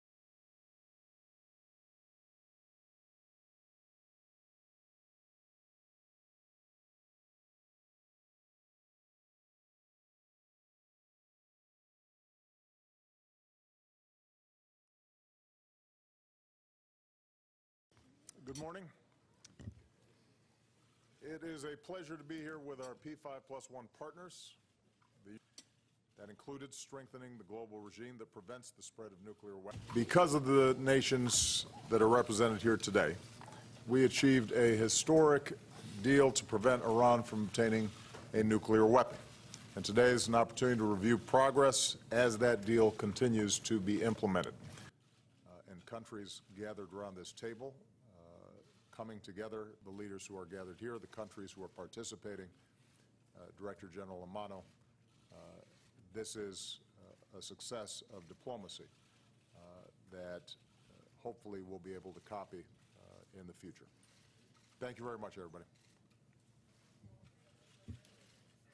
Leaders from The P5+1 Group, IAEA and EU Hold Talks on Iran Nuclear Deal
Chinese President Xi Jinping attended a meeting of the P5+1 group convened by U.S. President Barack Obama in Washington on Friday.

The leaders from the P5+1 group, namely China, France, Russia, the United Kingdom, and the United States, plus Germany, as well as the International Atomic Energy Agency (IAEA) and the European Union (EU), held talks on the Iran nuclear deal.

"Because of the nations that are represented here today, we achieved a historic deal to prevent Iran from obtaining a nuclear weapon. And today is an opportunity to review progress as that deal continues to be implemented," Obama said at the meeting.

British Prime Minister David Cameron, French President Francois Hollande, German Defense Minister Ursula von der Leyen, Russian Ambassador to the U.S. Sergey Kislyak, Director General of the IAEA Yukiya Amano, and EU President Donald Tusk attended the meeting.

The meeting took place on the sidelines of the Fourth Nuclear Security Summit (NSS) underway in Washington.

On July 14, 2015, Iran reached the Joint Comprehensive Plan of Action (JCPOA) with the P5+1 group consisting of the five permanent members of the UN Security Council, plus Germany, over its controversial nuclear program after more than 18 months of marathon talks.

The NSS, a biennial event initiated by Obama, draws together leaders and envoys from more than 50 countries and four international organizations this year, who will focus on advancing tangible improvements in nuclear security behavior and strengthening the global nuclear security architecture.